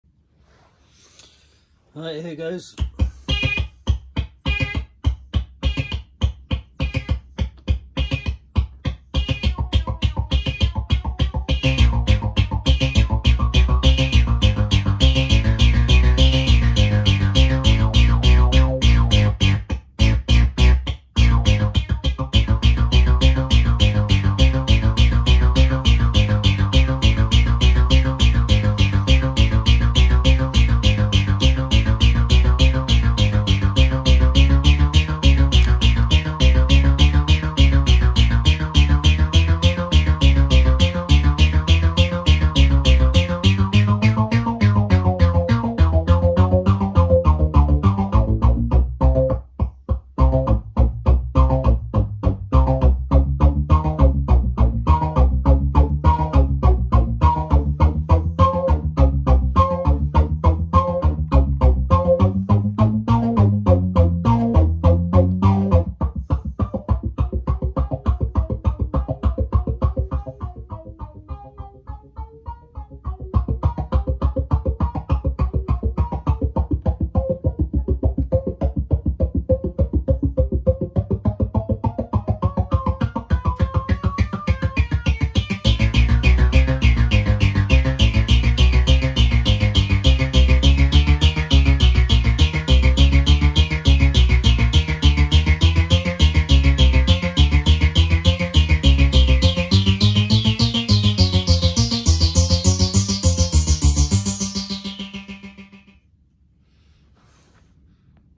The Isolator (my DIY modular synth with 2 Baby 10 sequencers): Blinky pitch shifts Clyde
A mostly hands off patch where Blinky the red baby 10 sequencer is pitch shifting Clyde the yellow Baby 10.